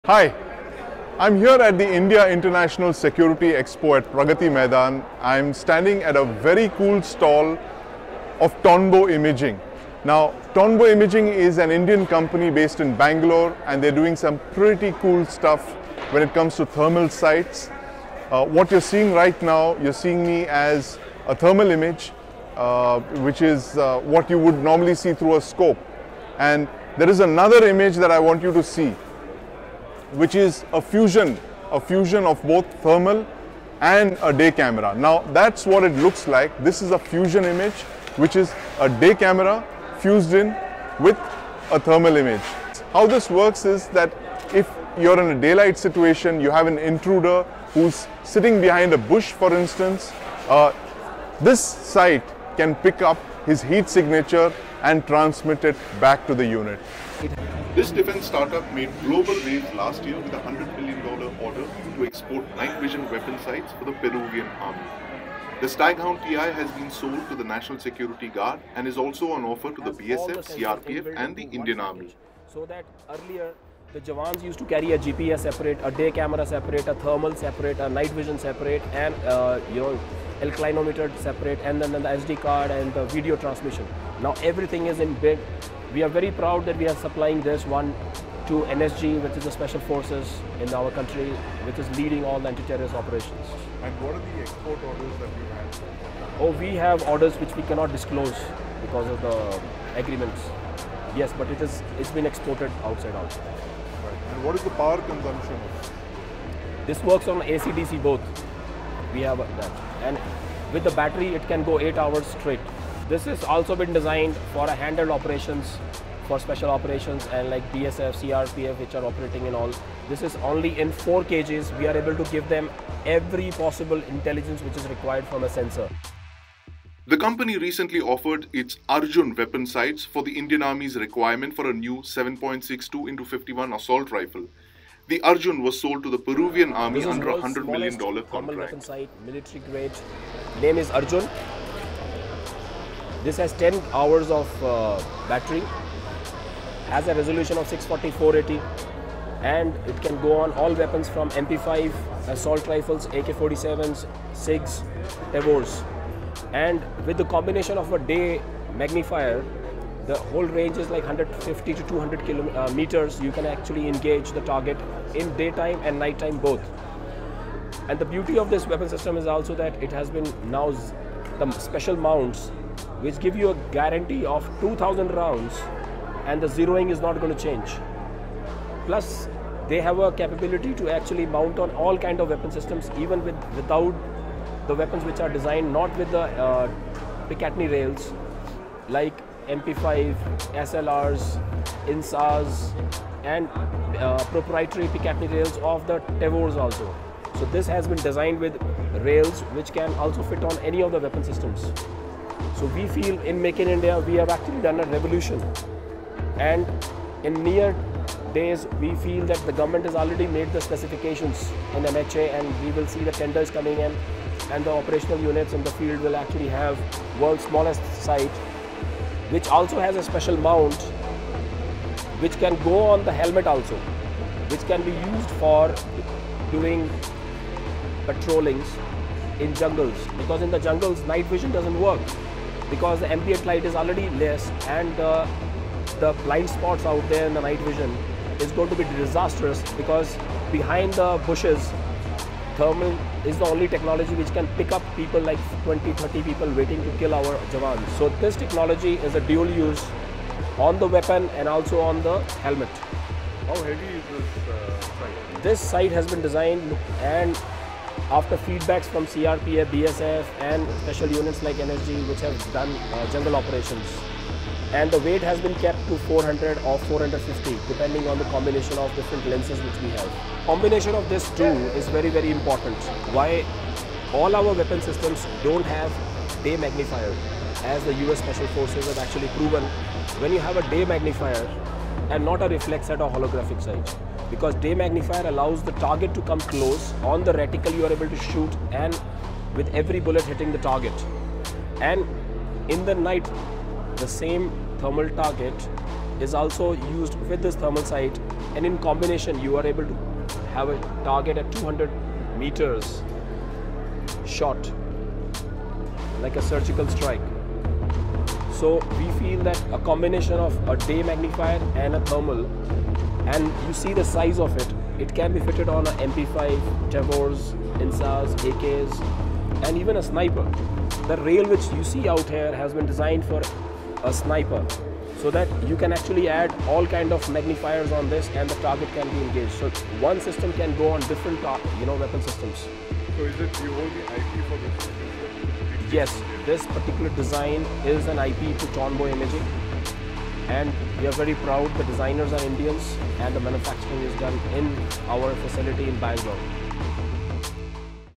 Thermal Imaging and Weapon sights for NSG, Indian Army
From a Bengaluru-based Indian start up that's making waves in the area of indigenous thermal and weapon sights. First orders placed by NSG recently. More orders expected from the other central police and paramilitary organisations.